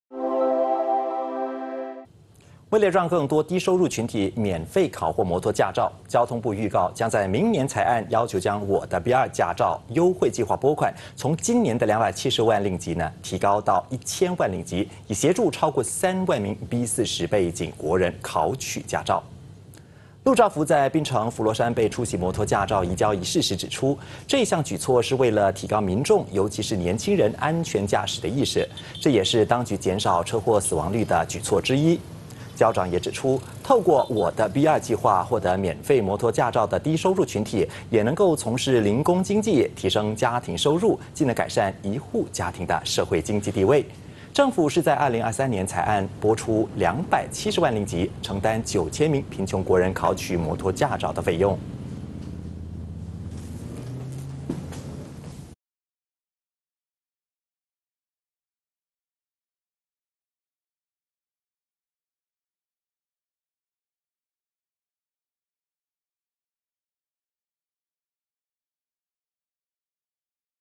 交通部申请增加拨款 助低收入者考B2摩托驾照【2023.07.08 八度空间华语新闻】
【交通部申请增加拨款  助低收入者考B2摩托驾照】
为了让更多低收入群体免费考获摩托驾照，交通部预告，将在明年财案要求将我的B2驾照优惠计划拨款，从今年的270万令吉，提高到1千万令吉，以协助超过3万名B40背景国人考取驾照。

陆兆福在槟城浮罗山背出席摩托驾照移交仪式时指出，这项举措是为了提高民众，尤其是年轻人安全驾驶的意识。这也是当局减少车祸死亡率的举措之一。

交长也指出，透过我的B2计划获得免费摩托驾照的低收入群体，也能从事零工经济，提升家庭收入，进而改善一户家庭的社会经济地位。政府是在2023年财案拨出270万令吉，承担9千名贫穷国人考取摩托驾照的费用。

【今日主播】叶剑锋 & 洪美玲